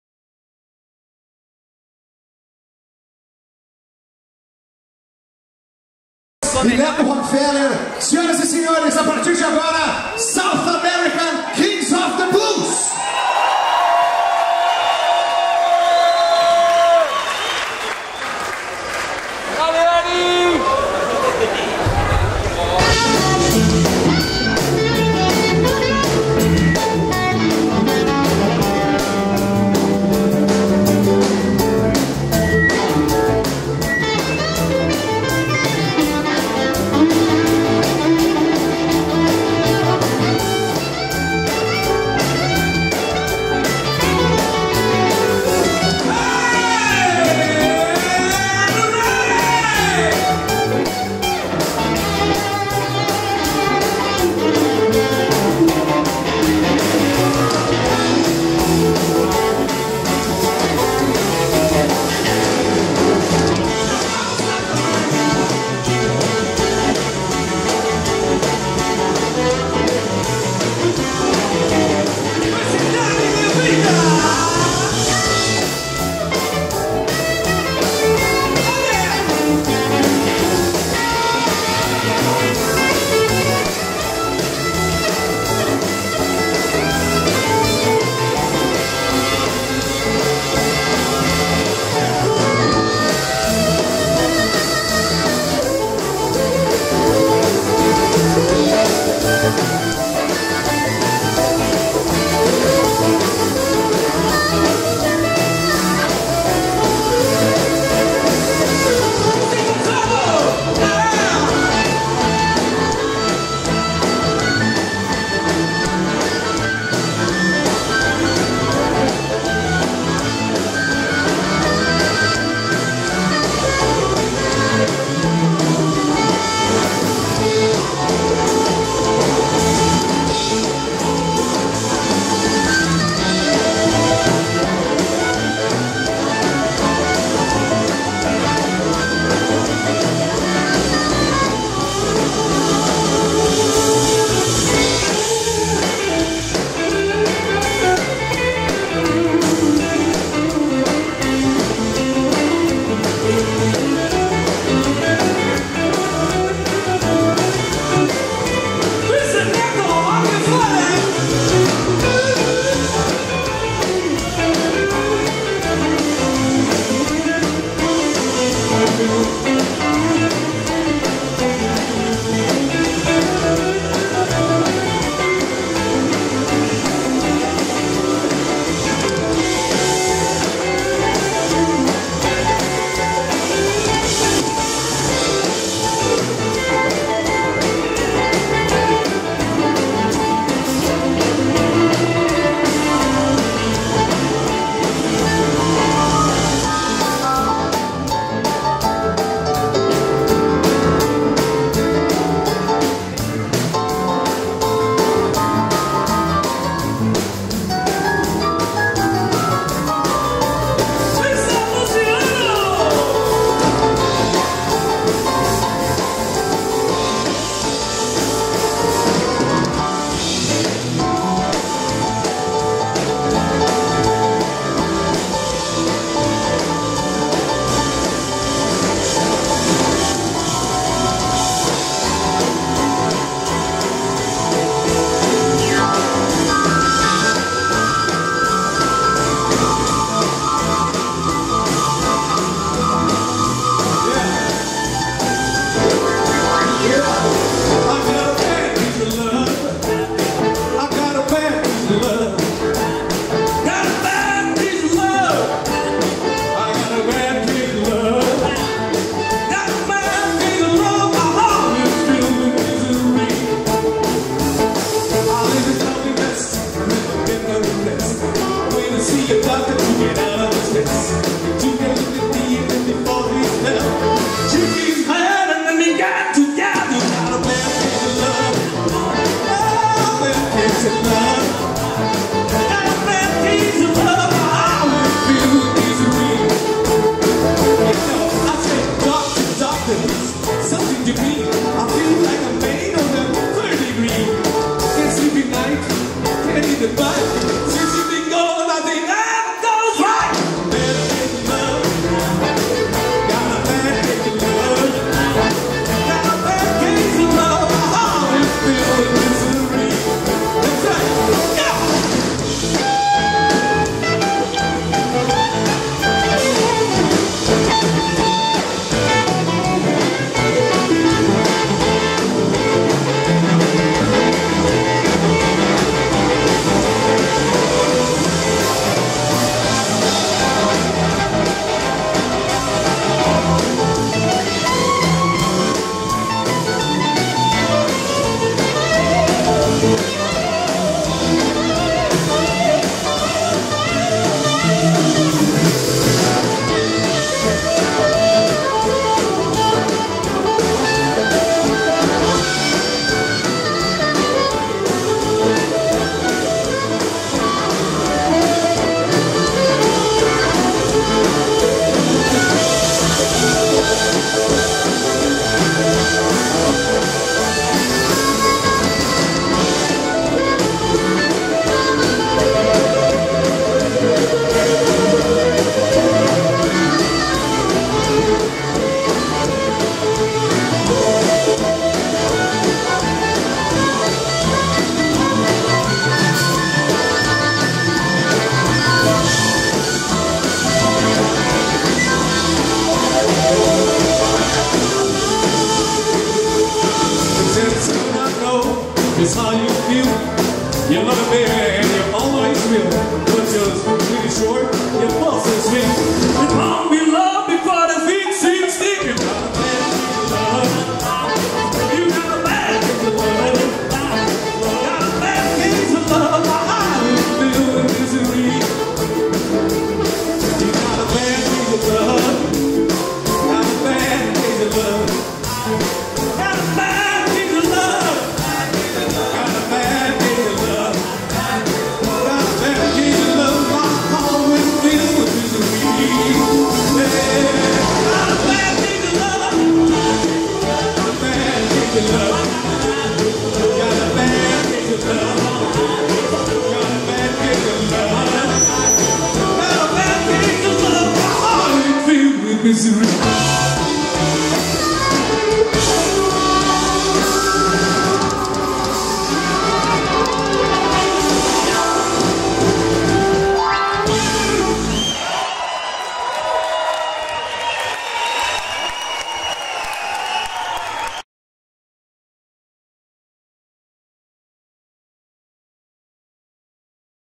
SOUTH AMERICAN KINGS OF THE BLUES.8ºMISSISSIPPI D.BLUES FEST.28-11-15.CAXIAS.By NEUSACALEGARI!
SOUTH AMERICAN KINGS
OF THE BLUES

--ANDY BOY SERRANO-(vocals)
(PORTO ALEGRE-RS-BRASIL);
--GONZALO ARAYA-(harmonica)
(CHILE);
--LUCIANO LEÃES-(keyboards)
(PORTO ALEGRE-RS-BRASIL);
--NETTO ROCKFELLER-(guitar)
(SÃO CARLOS-SÃO PAULO);
--DANIEL DE VITA-(guitar)
(BUENOS AIRES-ARGENTINA);
--FREDDY MUÑOZ-(bass)
(CHILE);
--PATO RAFFO-(drums)
(BUENOS AIRES-ARGENTINA).

8º MISSISSIPPI DELTA BLUES
FESTIVAL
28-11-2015
3º DAY
MOON STAGE.
LARGO DA ESTAÇÃO FÉRREA.
CAXIAS DO SUL-
RIO GRANDE DO SUL-
BRASIL.

RECORDING and EDITING:
By NEUSA CALEGARI